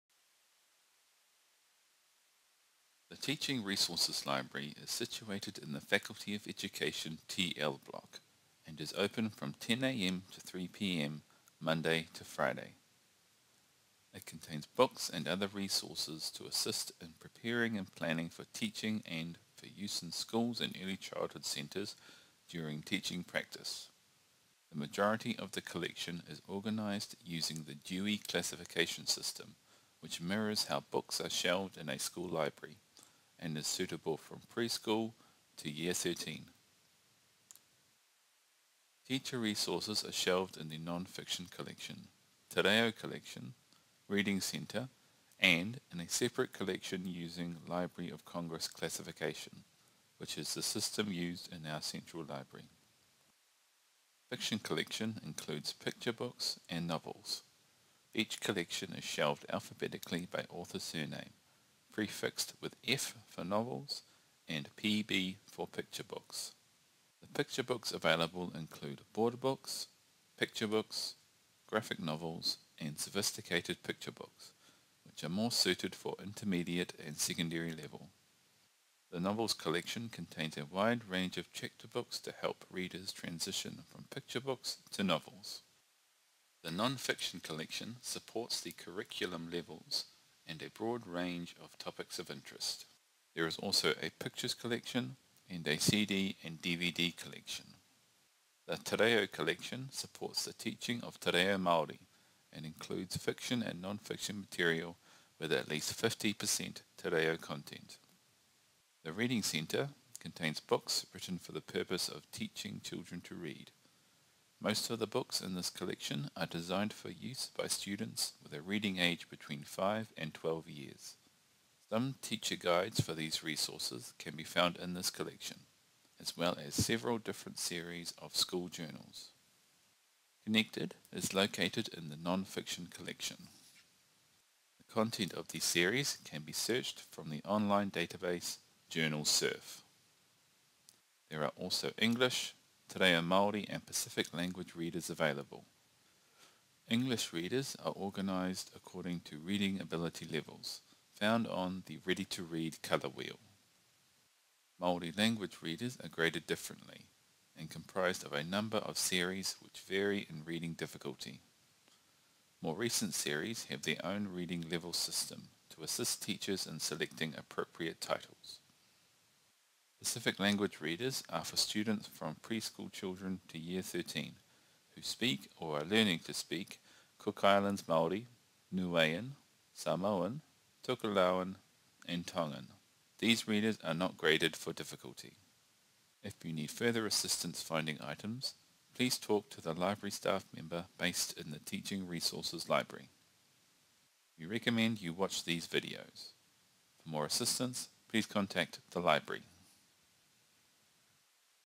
Using the Teaching Resources Library (TRL)
Learn about the different resources available in the Teaching Resource Library (TRL)